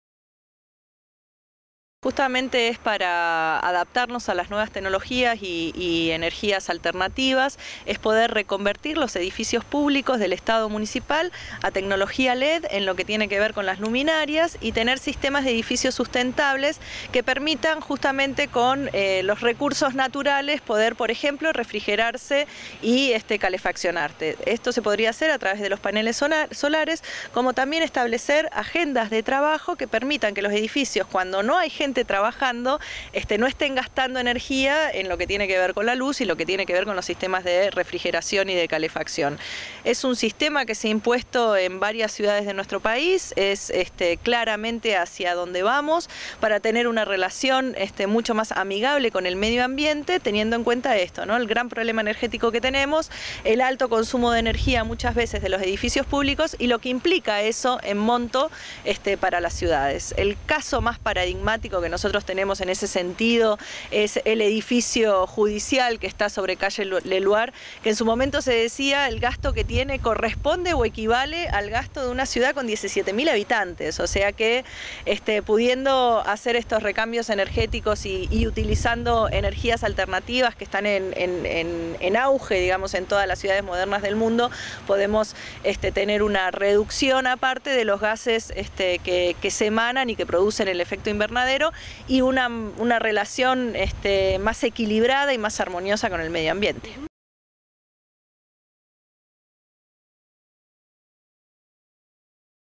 Neuquén: Proyecto Luminarias
Entrevista a Mercedes Lamarca y Cecilia Maletti,  concejales de Libres del Sur- Neuquén. Hace referencia al proyecto de Luminarias.